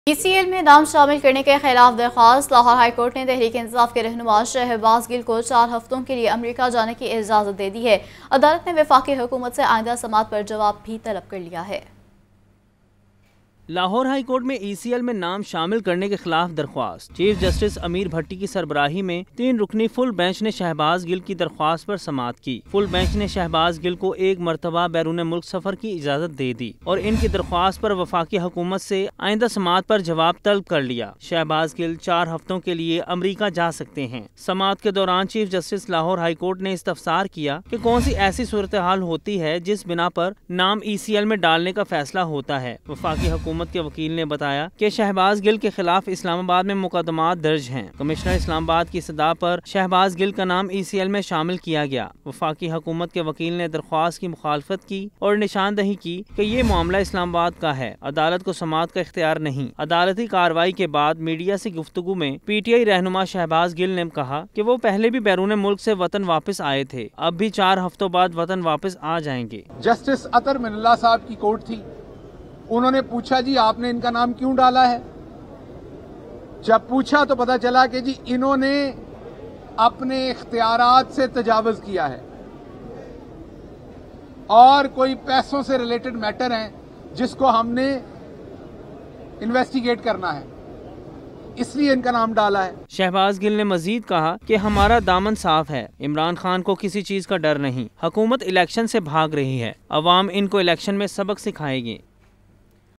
LHC Allows Shahbaz Gill To Pay Four Week Visit To US | Lahore News HD
The Lahore High Court (LHC) granted Pakistan Tehreek-e-Insaf (PTI) leader Shahbaz Gill permission on Wednesday to travel to the United States for four weeks.

A three-member bench headed by LHC Chief Justice (CJ) Muhammad Ameer Bhatti was hearing a petition seeking removal of Gill's name from the Exit Control List (ECL).

As proceedings commenced, CJ Bhatti questioned under what law the PTI leader's name had been placed on the list prohibiting him from leaving Pakistan.

The federal government's counsel informed the court that Gill had been accused in multiple cases in Islamabad and explained that he had been placed on the ECL upon commissioner Islamabad's request.